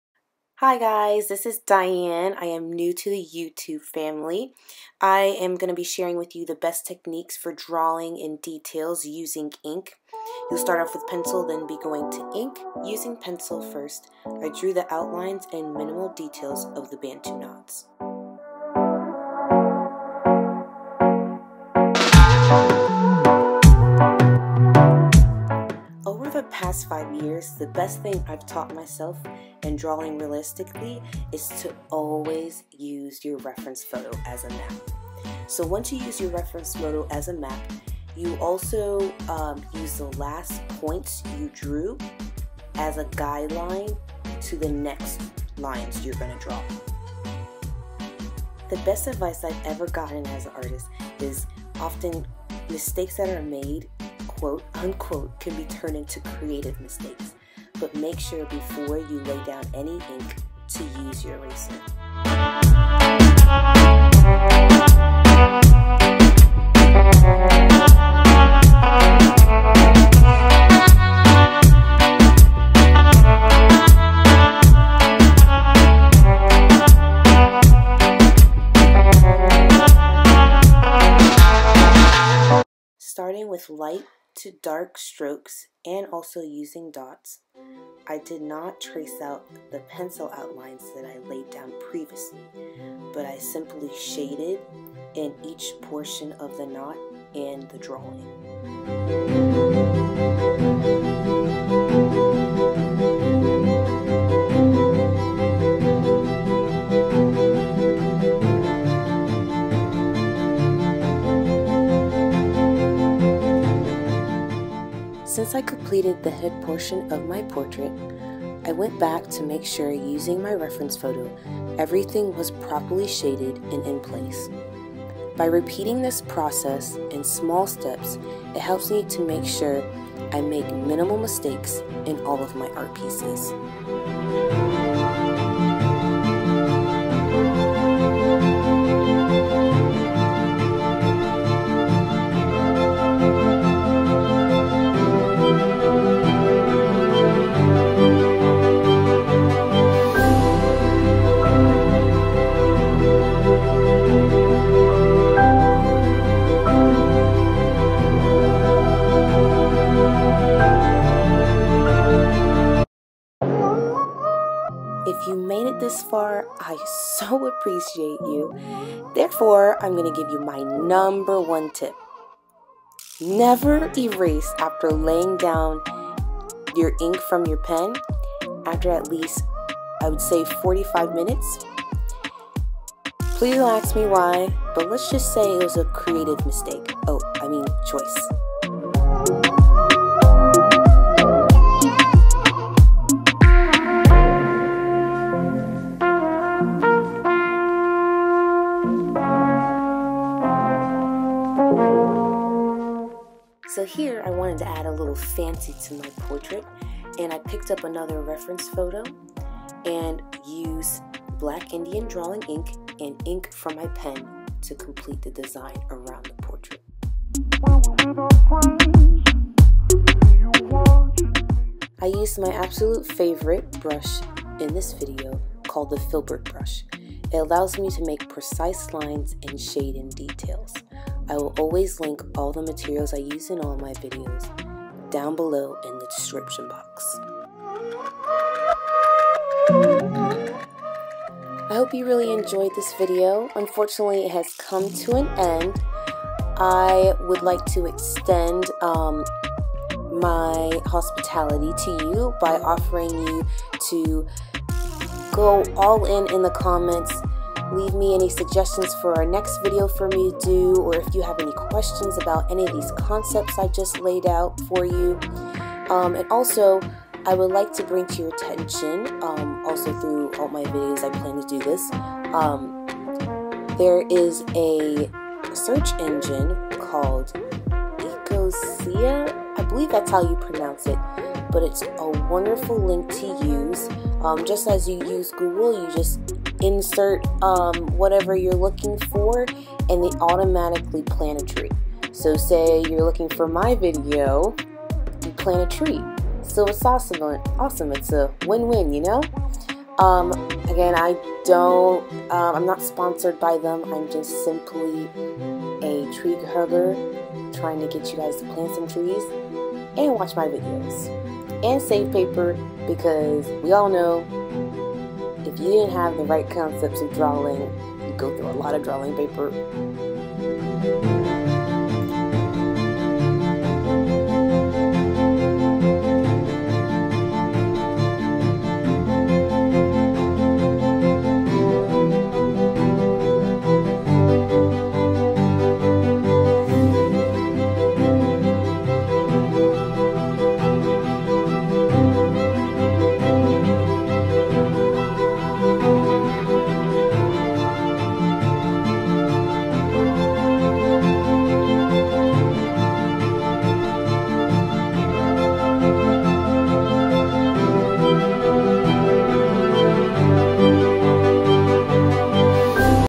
Best techniques for drawing details in ink: Bantu knots african american women.
Material

Pencil - 2 HB (not a mechanical pencil)
Pen Brand Pentel Slicci 0.25 (target)
Filbert brush size 4 Brand the fine's touch (Hobby Lobby)
Higgins Black Indian Ink
Water
Drawing paper Brand Startmore 18 by 24 series 400 count 24 (warm paper tone)